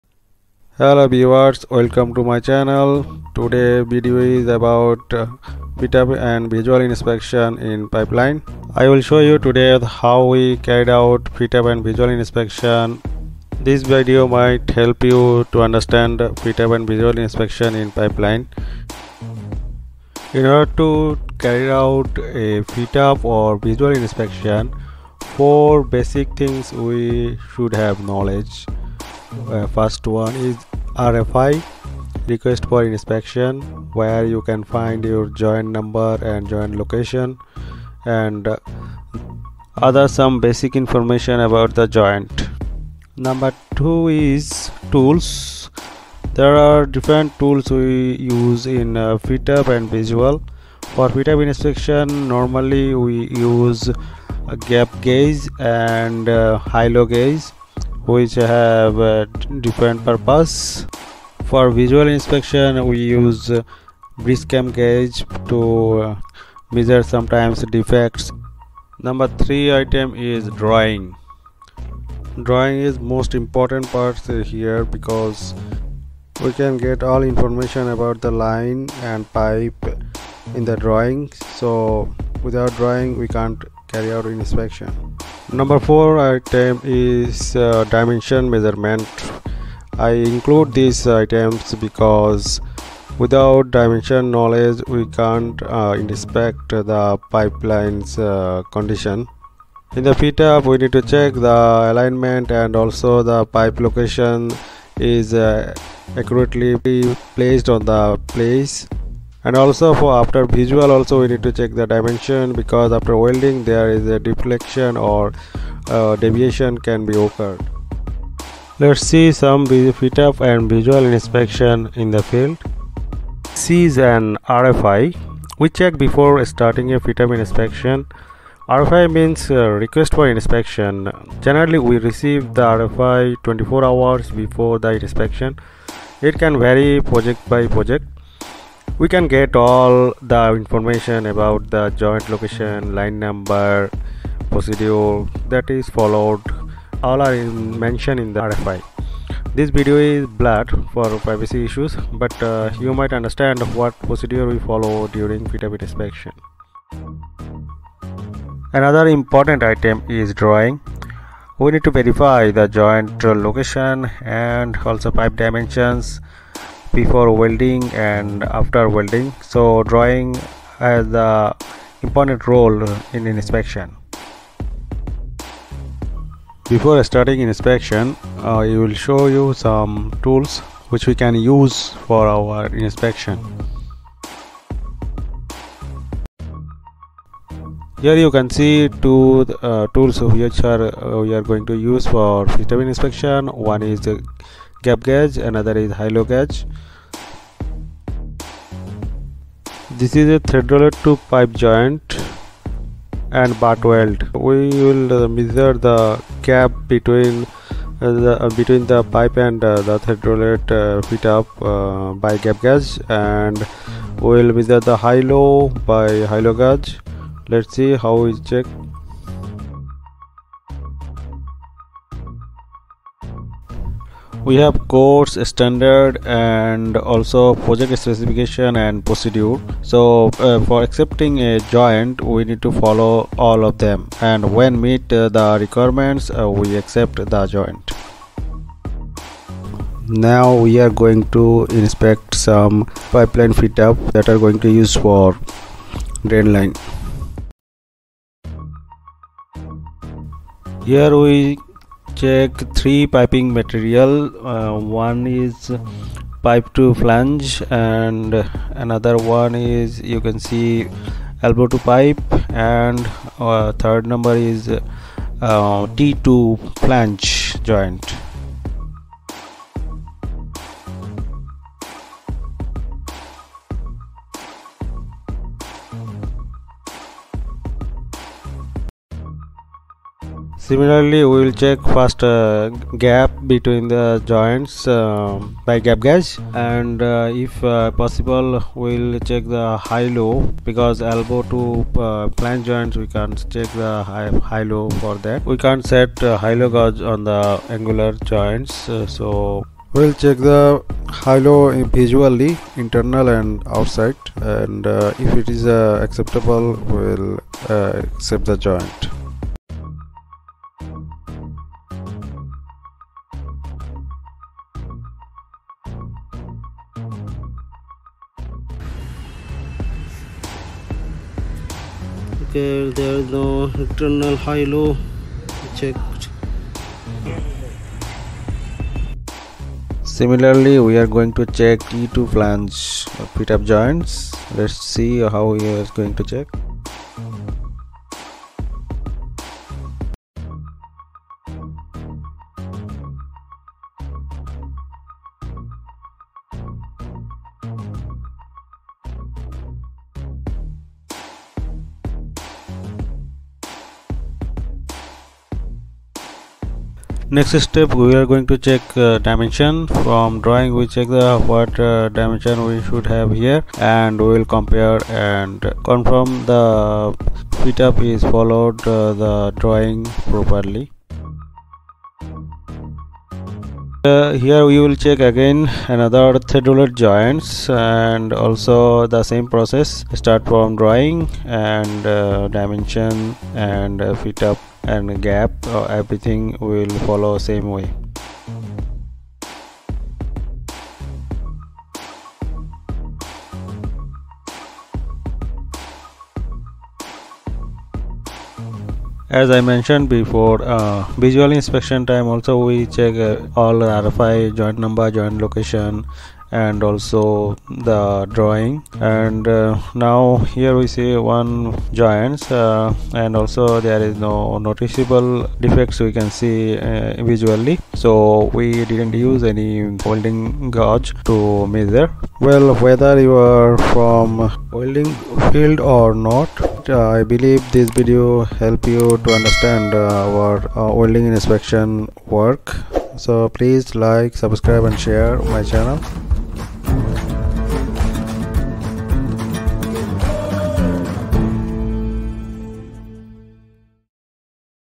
Pipe fit up & welding visual inspection Pipeline Joint fit up & visual Inspection verify welding
Pipe fit up & welding visual inspection| Pipeline welding fit up & visual Inspection |Welding Fit up & visual verification
What steps need to be followed to repair pipeline welding? How are RT welding defects located & removed